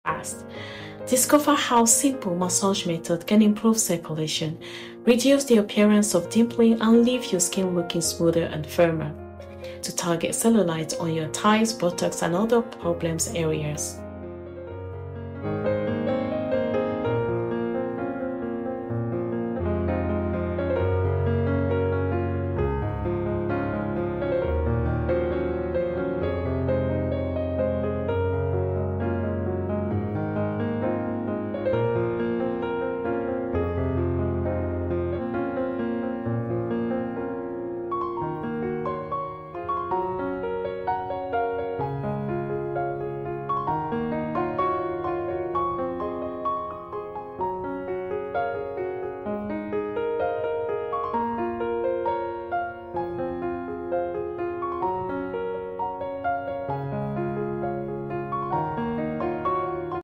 Do Anti-Cellulite Massages ACTUALLY Work?
Tips for effective anti-cellulite massage

When working on the buttock area, it's important to always lift the buttock to avoid gravitational ptosis. This technique helps maintain a youthful and firm appearance.

Next, let's try the pinching technique. Grab a small section of skin between your fingers and gently squeeze. You can do this in a rhythmic motion, moving along the thigh.

Remember, it's all about being consistent with these movements. The more you do it, the better the results